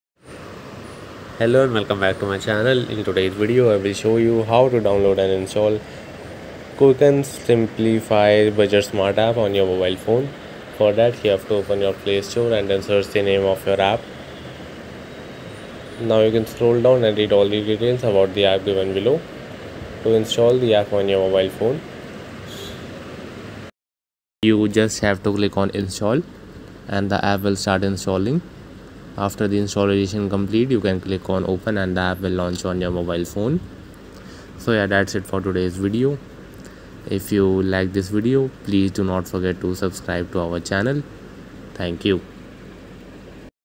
✅ How to Download and Install Quicken Simplifi Budget Smart App (Full Guide)
In this video, we provide a comprehensive guide on how to download and install the Quicken Simplifi Budget Smart app on your device. Follow along to successfully set up the app.

Key Learning Points:
• Step-by-step download process
• Installation tips
• Initial setup